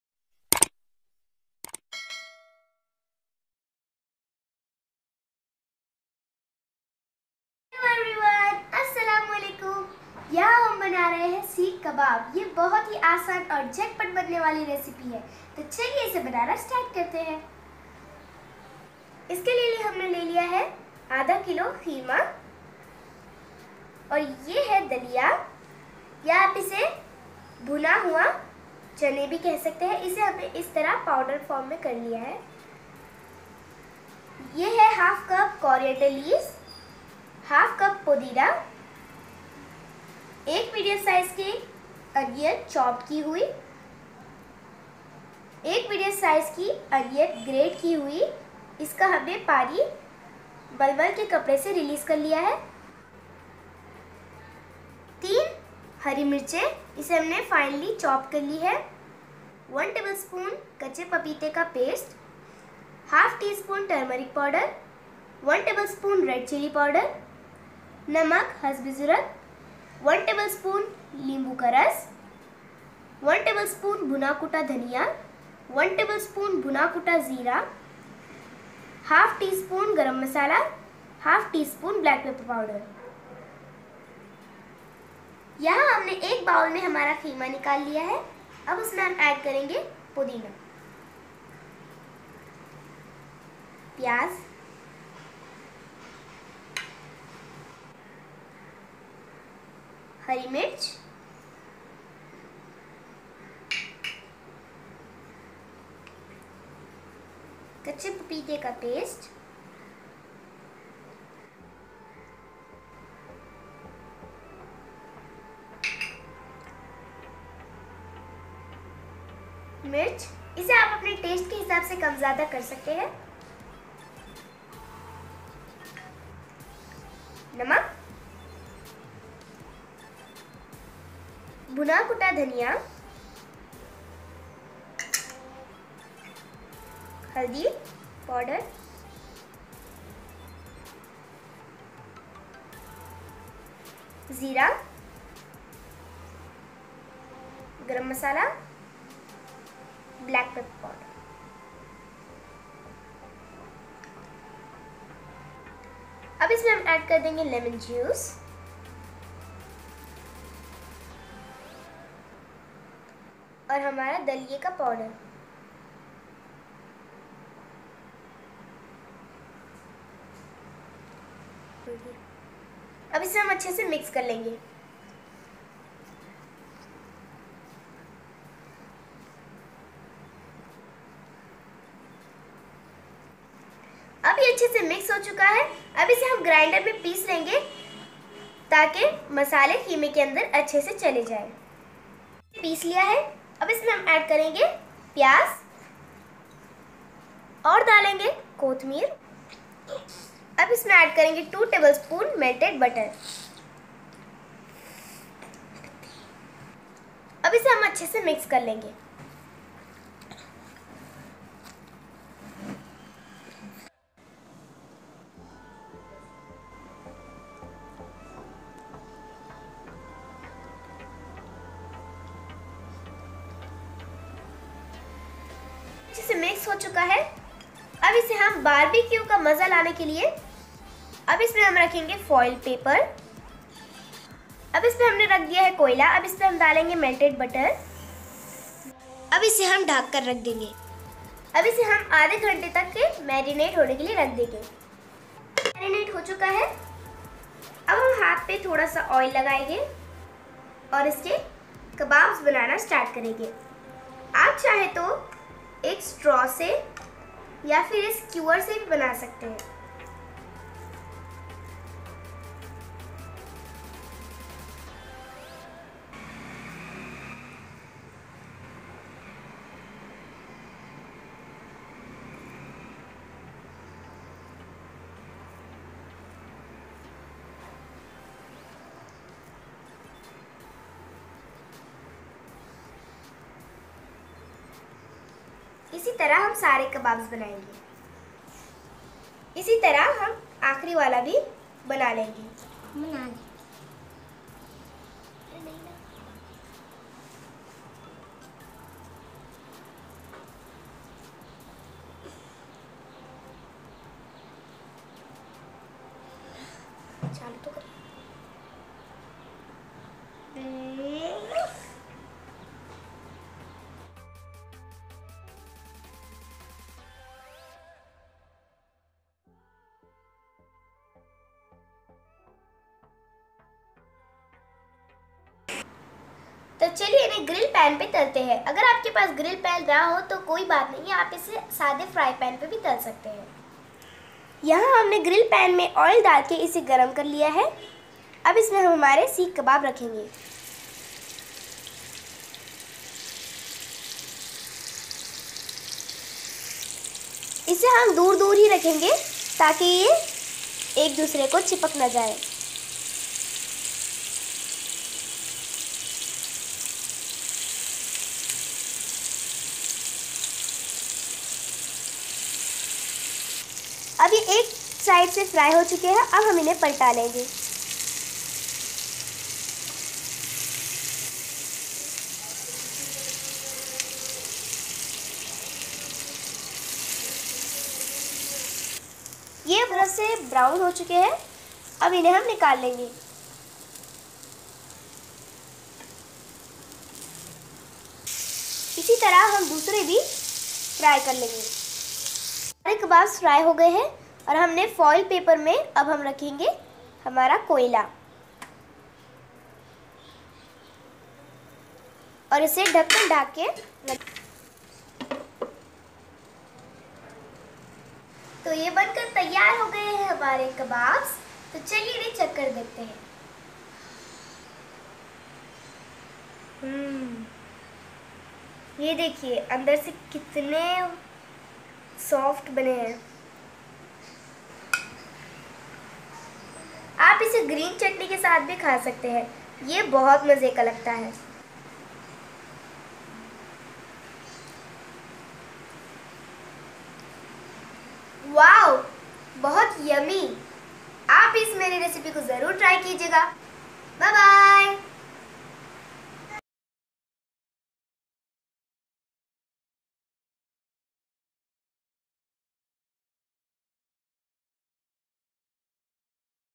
Sheekh kabab recipe || kabab recipe 2020 || Sheekh kabab by Sabrina's kitchen...
Sheekh kabab is a restaurant recipe and this recipe we have maded at home for you please try this recipe and enjoy my recipes please subscribe to my channel for more delicious recipes
hello everyone here is the

INGREDIENTS:

1/2 kg  - mince
1/2cup - podina
1/2cup - coriender leaves
4tbsp - daliya powder
1 chopped onion
1grated onion
3 green chillis
1 tbsp - red chilli powder
salt as per taste
1/2 tsp - turmeric powder
1tbsp - dry papaya paste
1 tbsp - lemon juice
1tbsp - red chilli powder
1tbsp - dry roast dhaniya
1tbsp - dry roast jeera
1/2 tsp - garam masala
1/2 tsp - black pepper powder

BYE BYE..